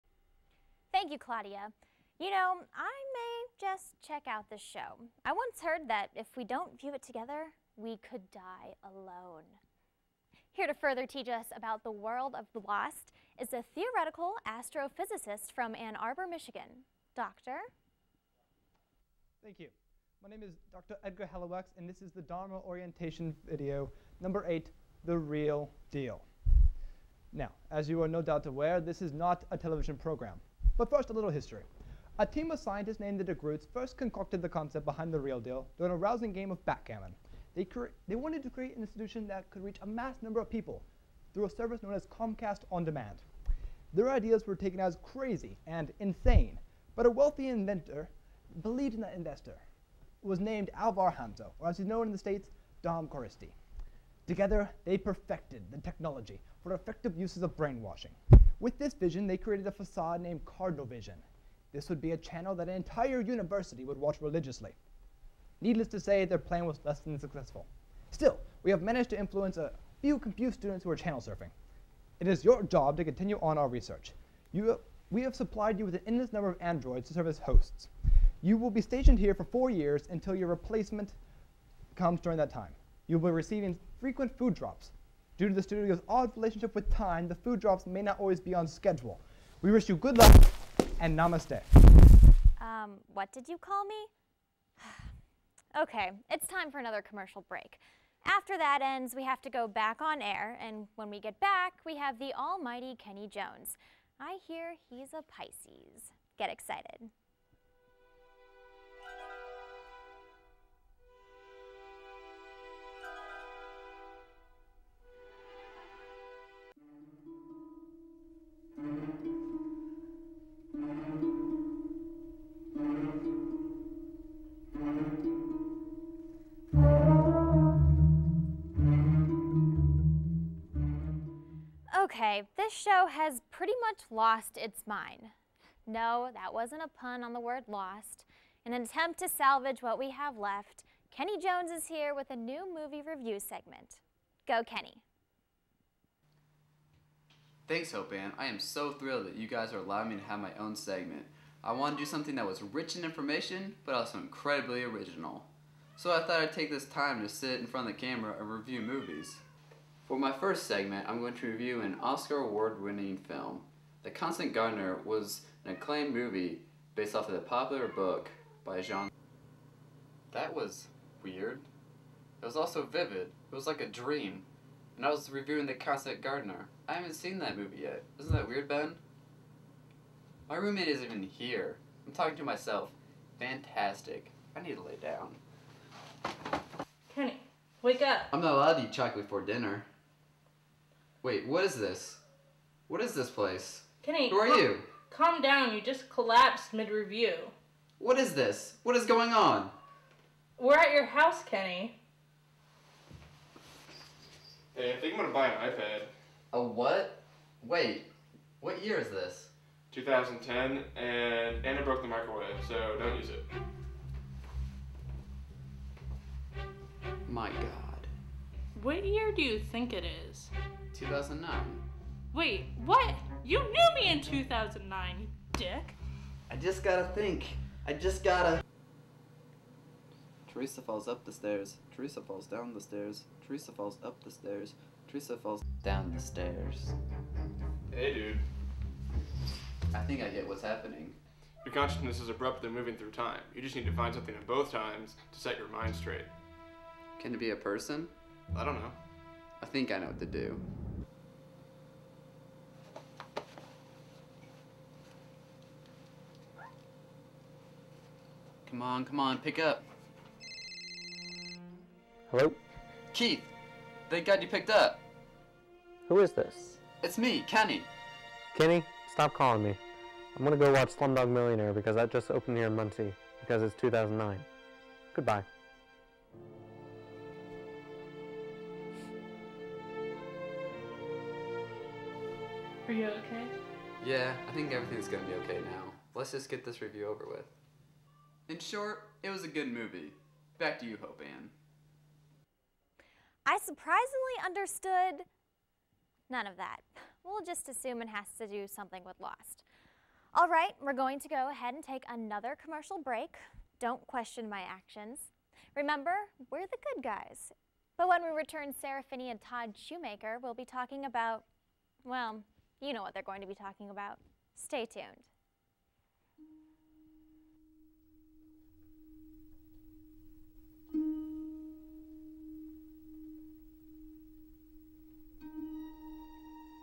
Reel Deal gets LOST Part 2 of 3
In honor of the start of the final season of LOST, Reel Deal opted to do a LOST themed episode. Trust us, everything in this episode is LOST. Part 2 of 3.